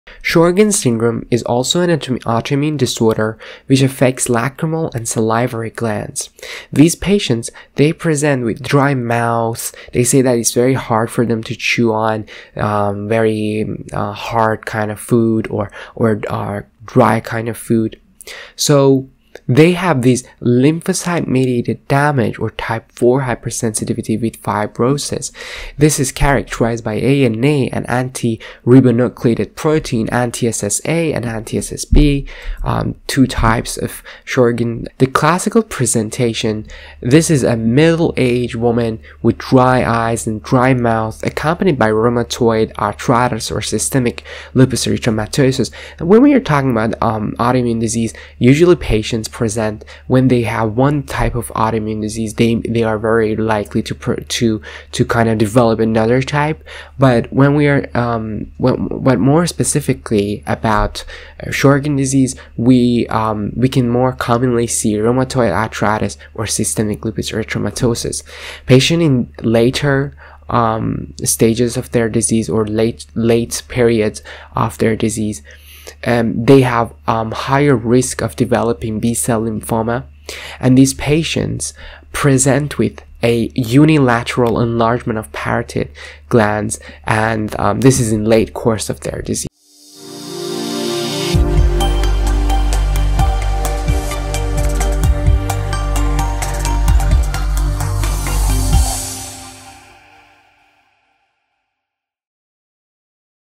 Sjogren's syndrome - Pathology
Pathology tutorial on Sjogren's syndrome

In this lesson you will learn:
- Symptomes of Sjogren's syndrome
- Lab studies of Sjogren's syndrome